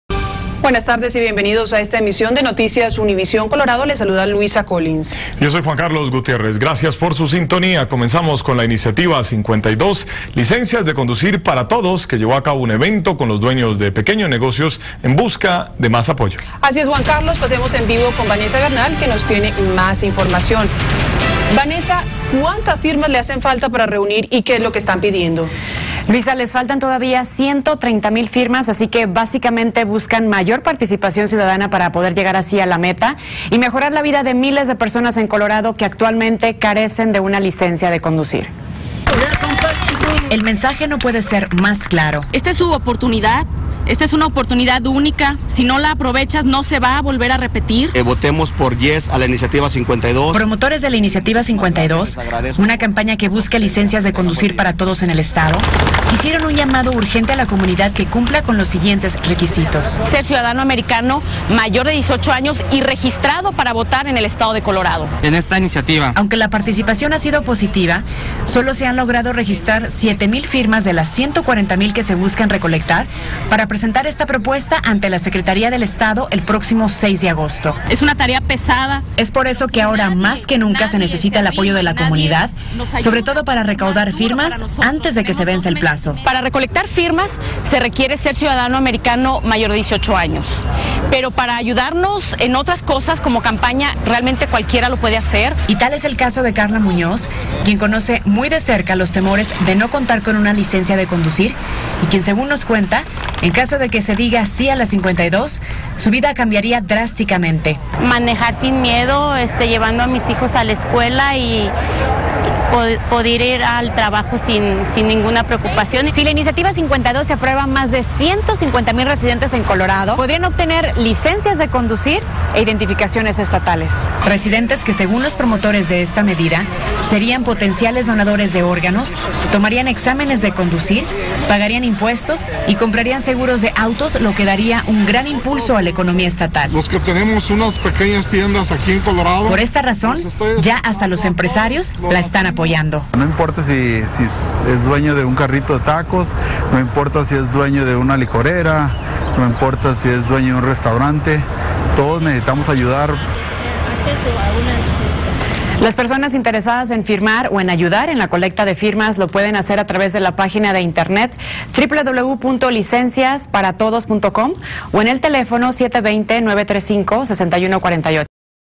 Licencias para todos en Colorado
La campaña conocida como licencias de conducir para todos o iniciativa 52, lanzó un nuevo empujón hoy para intentar conseguir la cantidad necesaria de firmas que lleve la propuesta a la boleta electoral el próximo mes de diciembre.